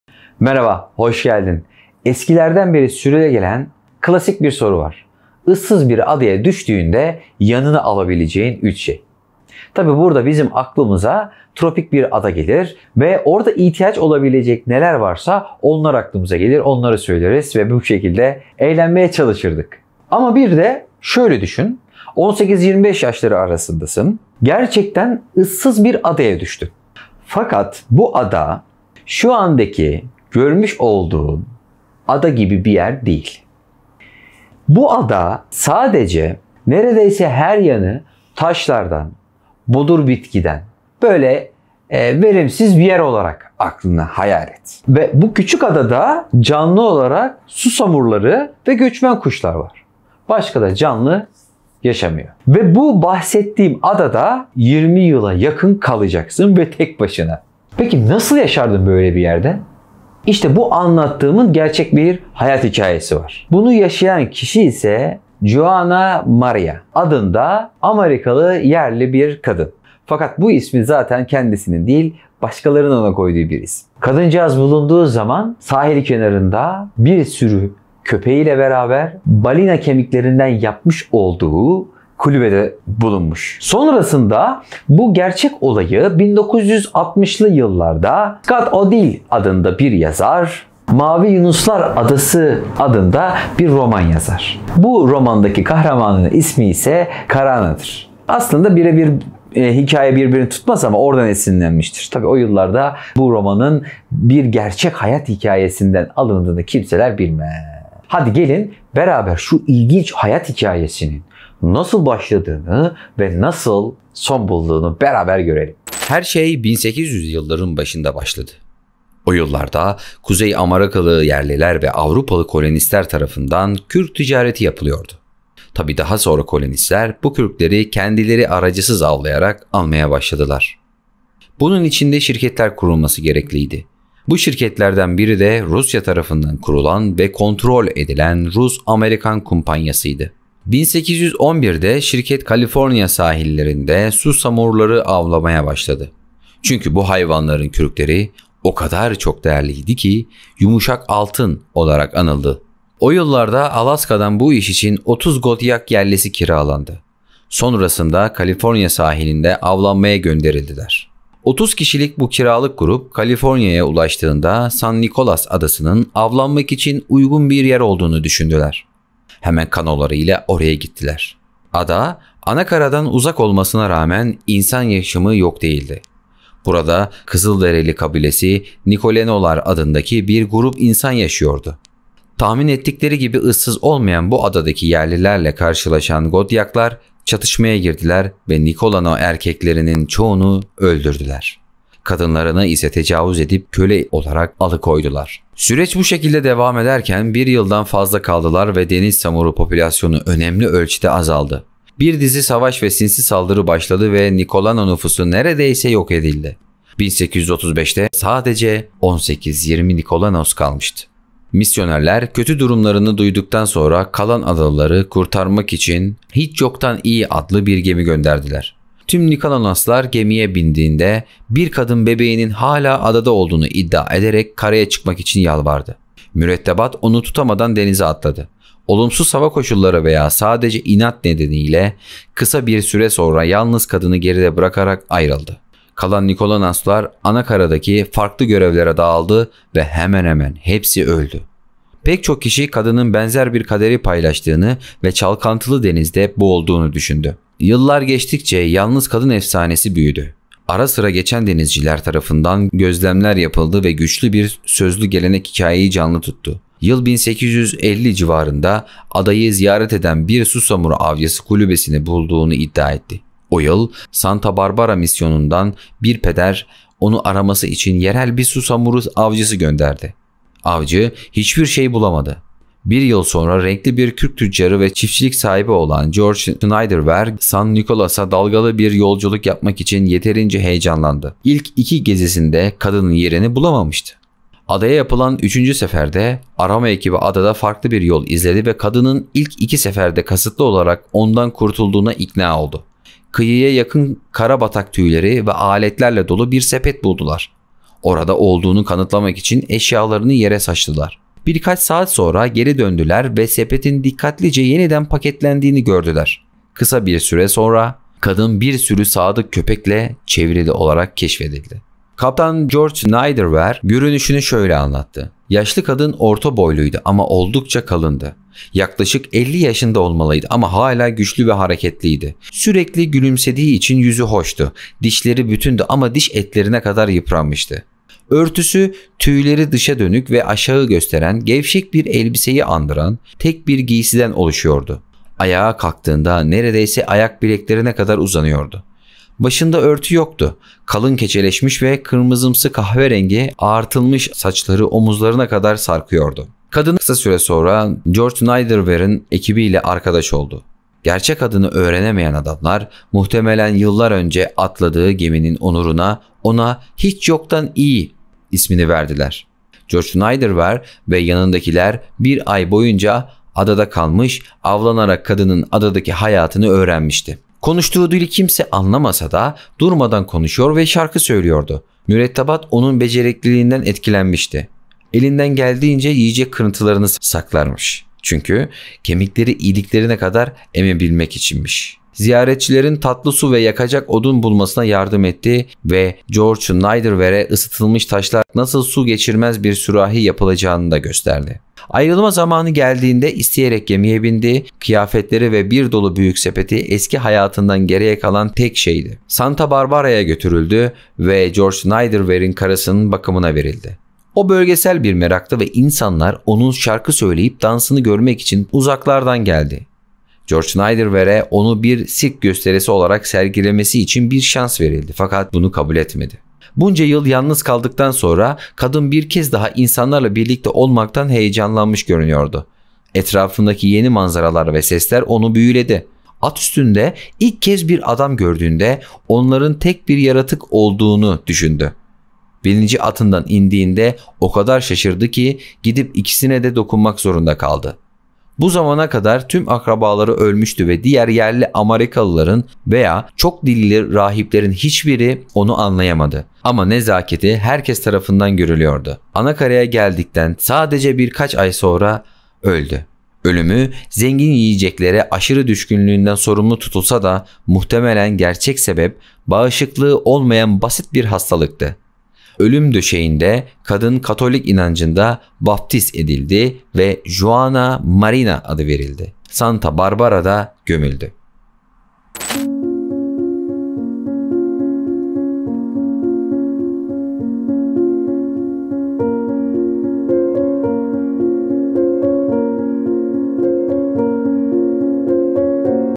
20 yıl ıssız adada mahsur kalan juana maria'nın hikayesi.ilginç olay belgesel
Merhaba;
1800'lü yıllarda San Nicolas adasında 20 yıla yakın mahsur kalan juana maria'nın inanılmaz hikayesi. Issız ve taşlarla kaplı olan bu adada yaşam şavaşıyla mücadele eden bu kadının hikayesini beraber öğrenelim.

iyi seyirler,